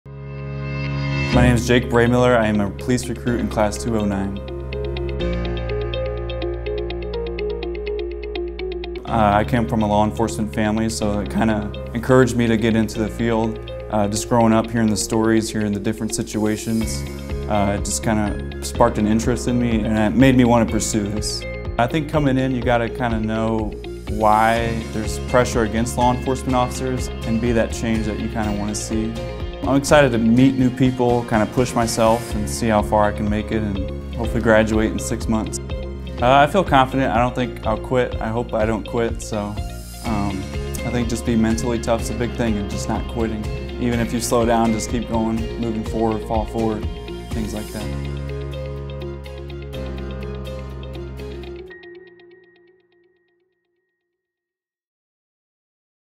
Meet the Police Recruits of Class 209: Jake Braymiller
Meet Jake Braymiller, an aspiring police recruit currently enrolled in the St. Louis County and Municipal Police Academy's Class 209.